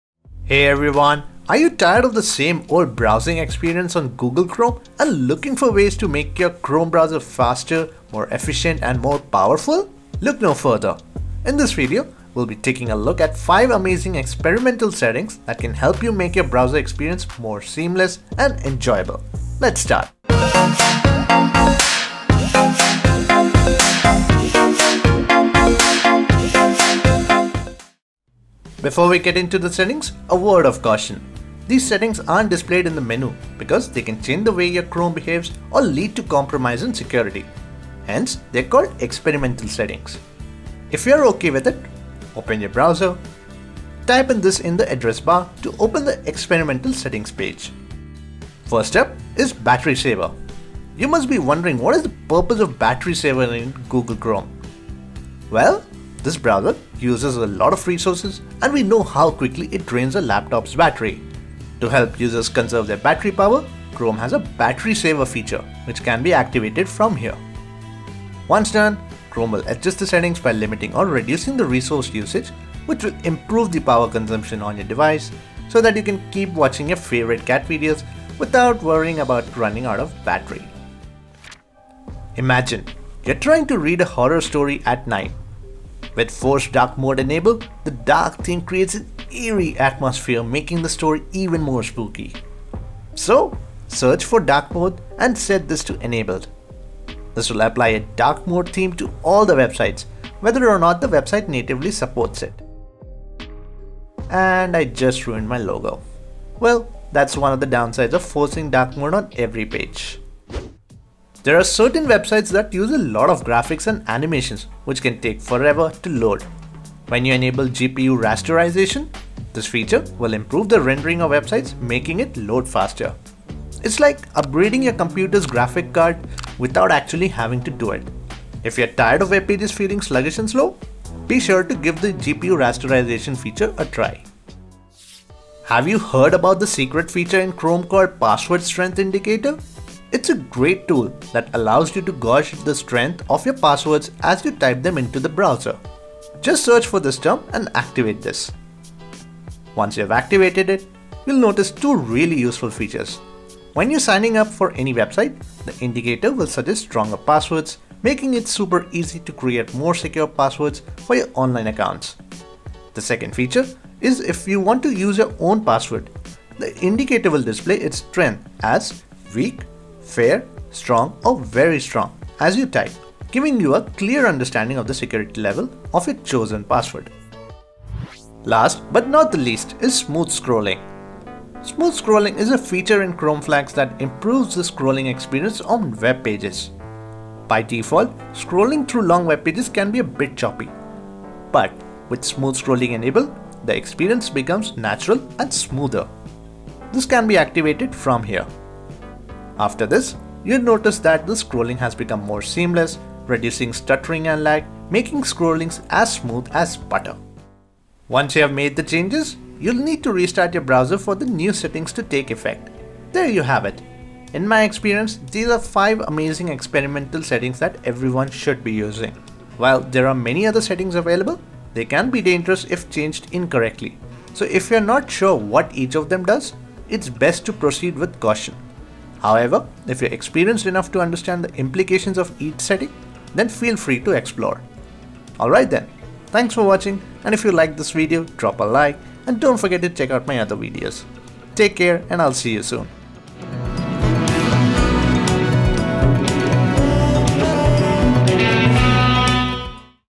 5 Amazing Experimental Settings To Unlock The Power Of Chrome! | 2023
🌐 In this video, I reveal 5 Amazing Experimental Settings hidden within Google Chrome such as forcing dark mode on every webpage, display password strength indicator to make stronger password during sign up, etc. Learn how to access these hidden settings and see the difference they can make to your online experience.

🕒 Click on the Timestamp below which will direct you to the section of a video that might be right for you:

00:00 - Intro
00:28 - Caution
00:48 - Battery Saver
01:51 - Force Dark Mode
01:41 - GPU Rasterization
02:16 - Password Strength Indicator
02:59 - Smooth Scrolling
03:28 - Word of Advice
03:55 - Outro